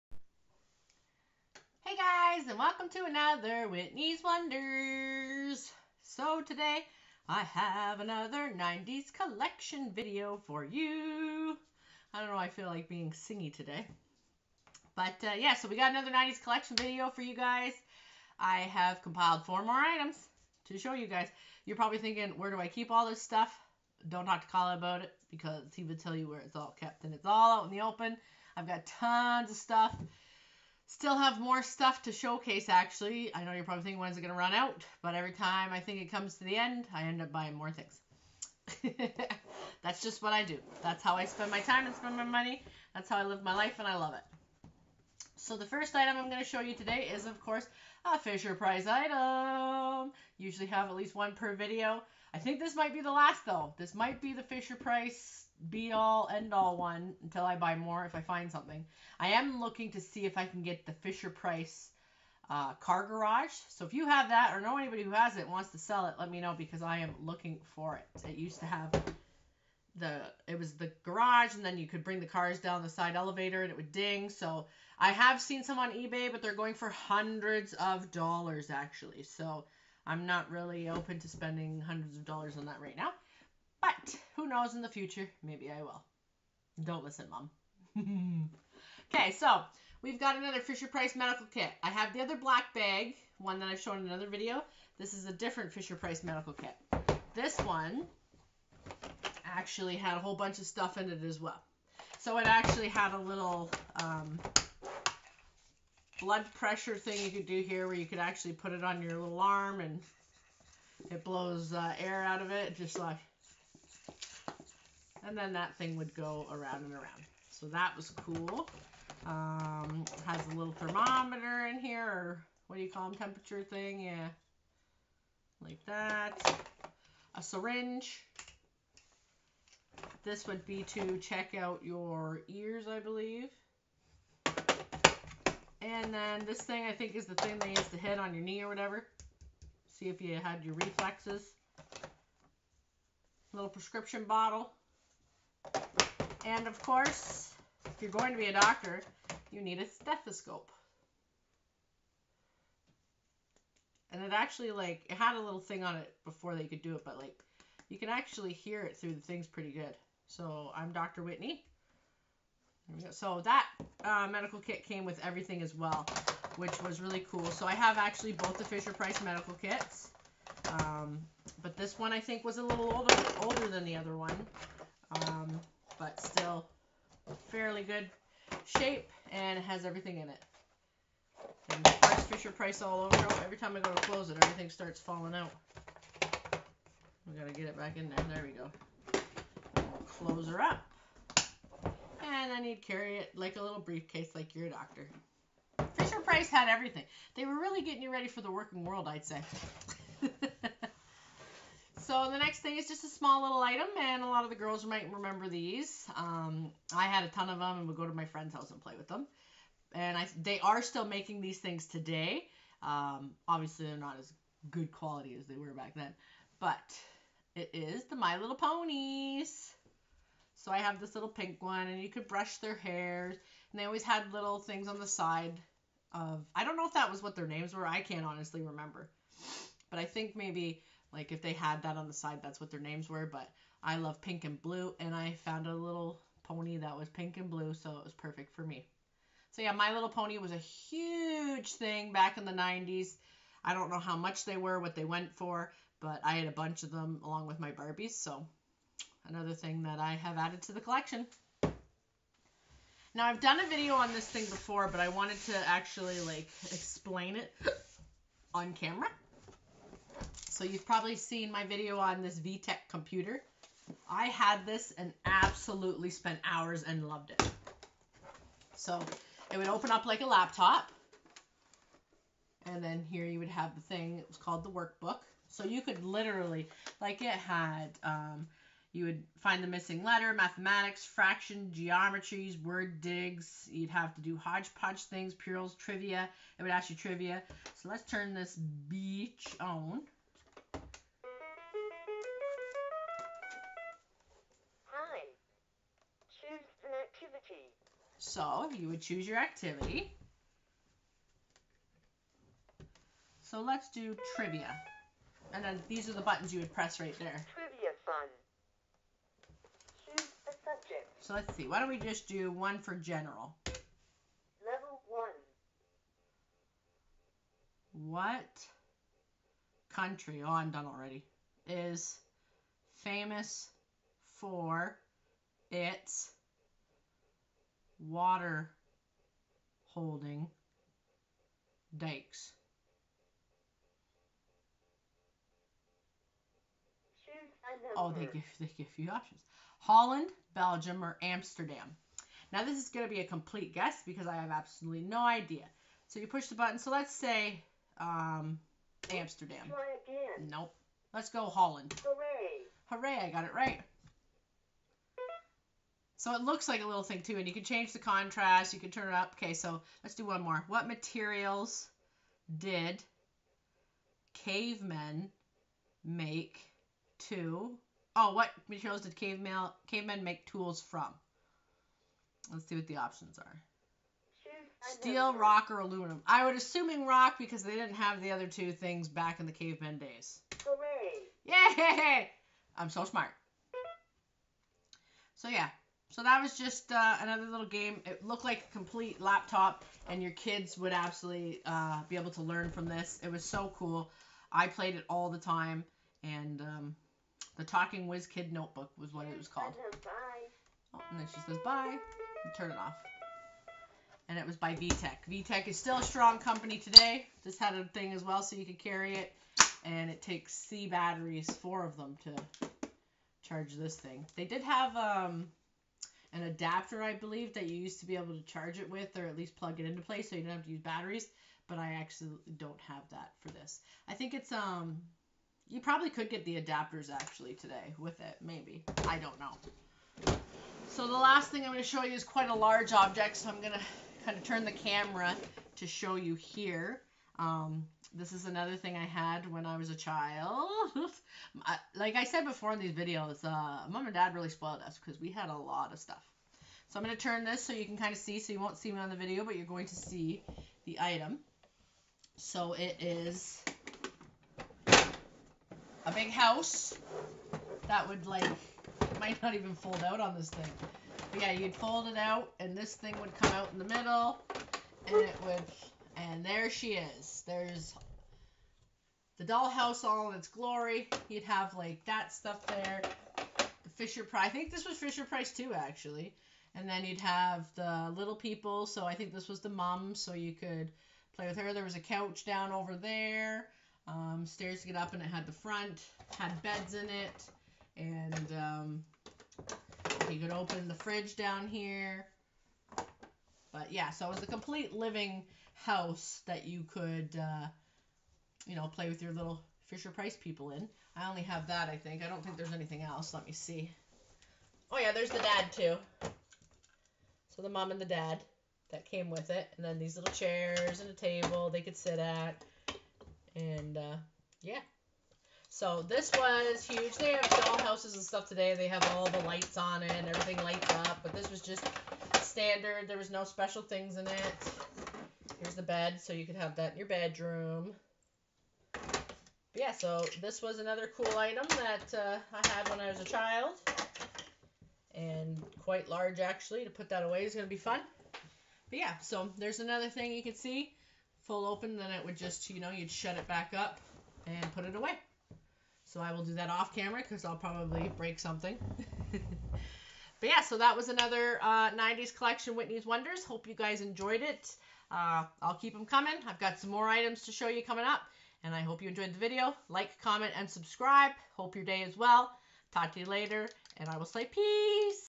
90's Collection
Showcasing more of my collection! My Fisher Price Medical Kit, My Little Pony, Vtech Talking Whizkid Notebook and my Dollhouse!